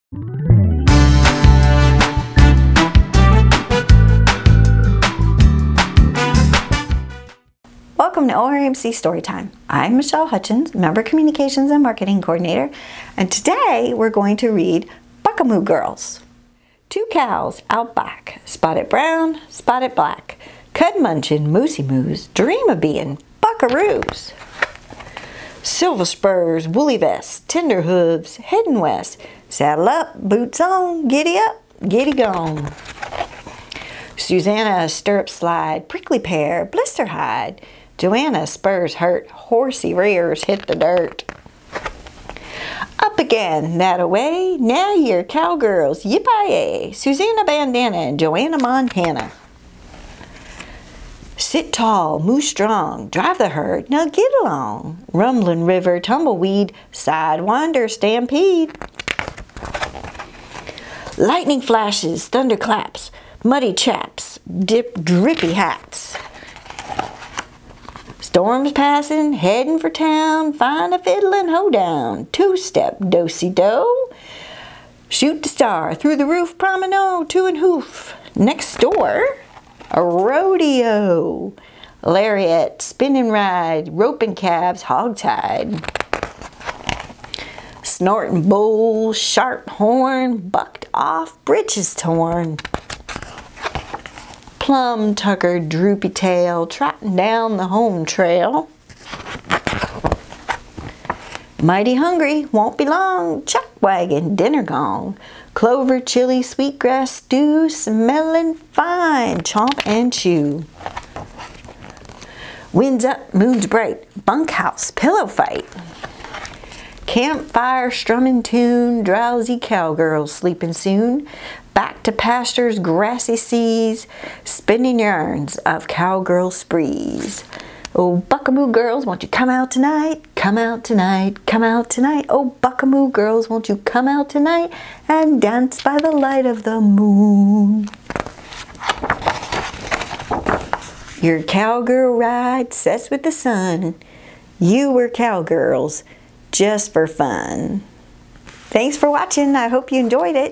OREMC Storytime: Buckamoo Girls
OREMC Member Communications/Marketing Coordinator Michele Hutchins reads Buckamoo Girls. What would your buckaroo name be? How many cactuses do you see? Have you ever been to a rodeo? What campfires songs do you know?